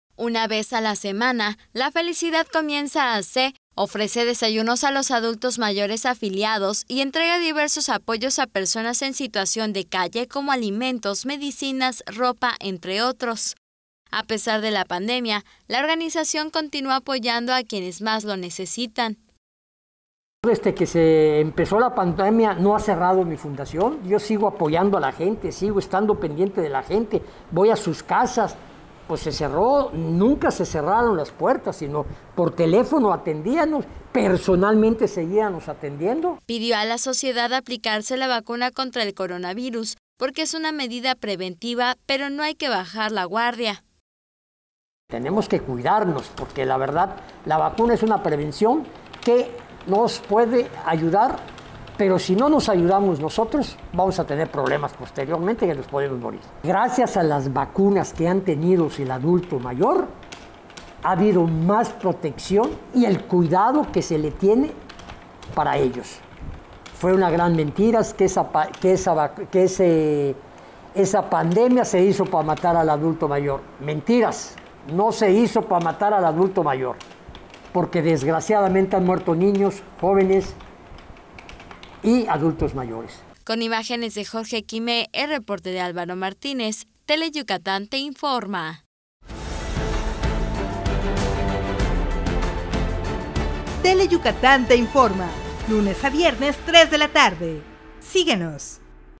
❤️Esta asociación apoya a adultos mayores en situación de calle❤️ | TELE Yucatán
Una organización de mucho corazón ❤️ ofrece desayunos a los adultos mayores y diversos apoyos a la gente en situación de calle en Mérida
¡Conoce la historia! 👏🏻👏🏻